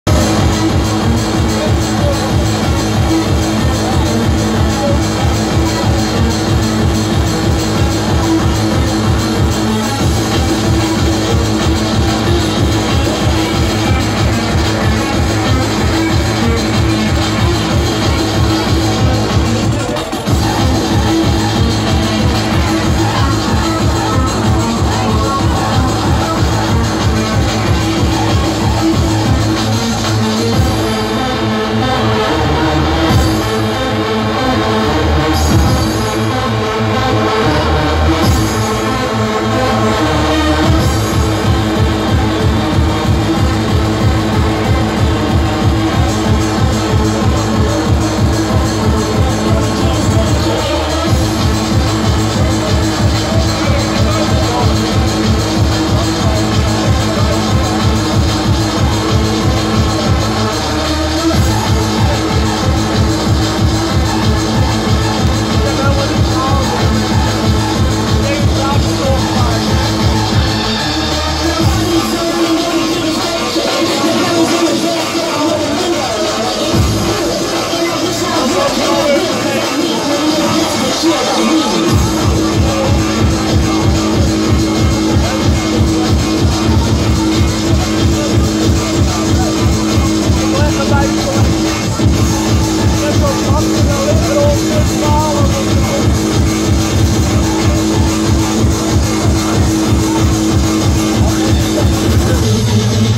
Predator @ Masters of Hardcore 2023
Predator closed the early stage of Masters of Hardcore.

Location: Masters of Hardcore - Cosmic Conquest. Saturday the 25th of March 2023 Brabanthallen Den Bosch, the Netherlands